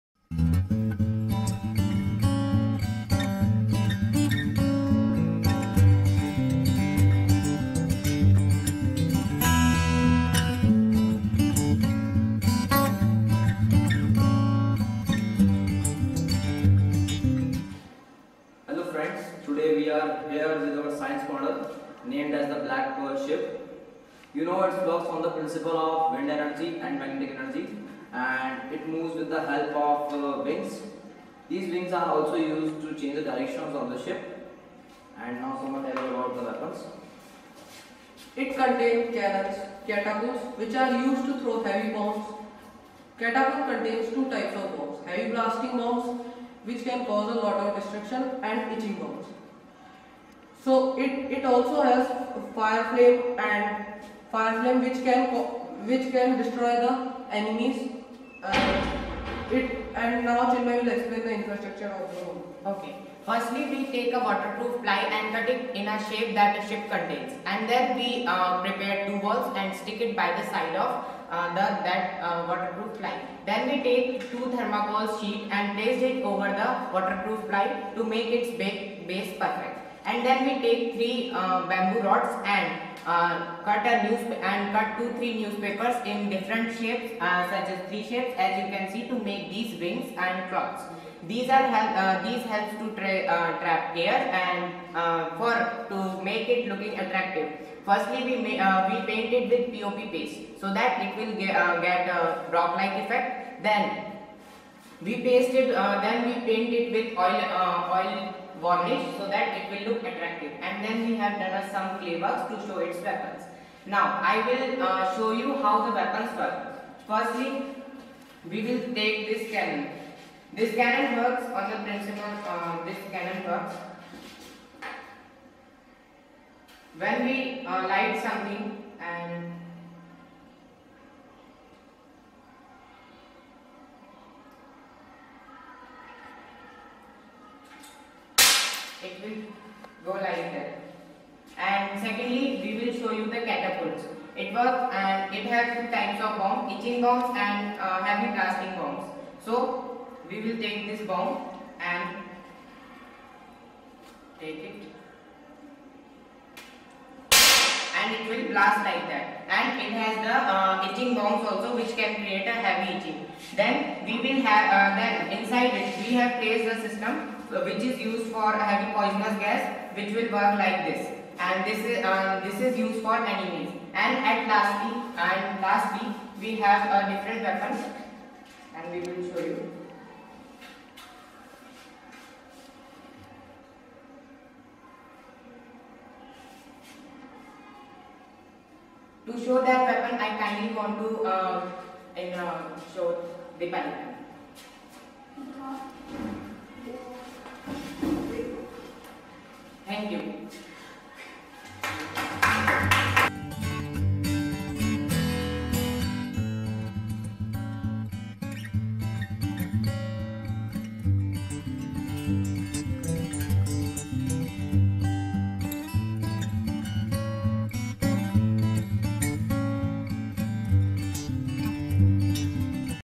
St. Paul's School, Baran [Science Project]
Physics Dimensions Project made by Class 9th Students.